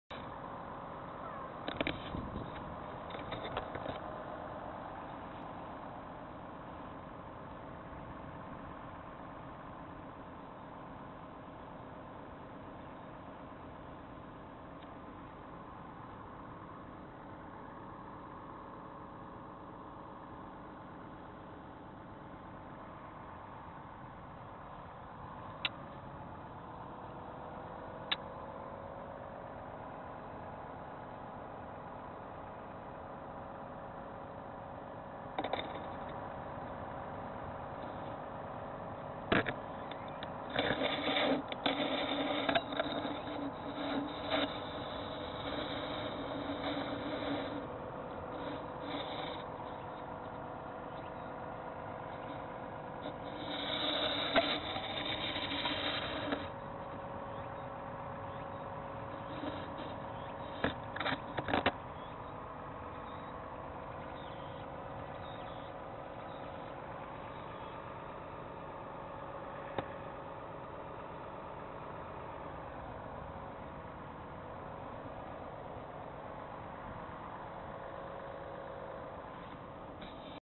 Two Japan stray Brown tabby cats.
Summer of Japan.
This is mediocrity suburb Japanese scenery.
To people who are interested in Japan.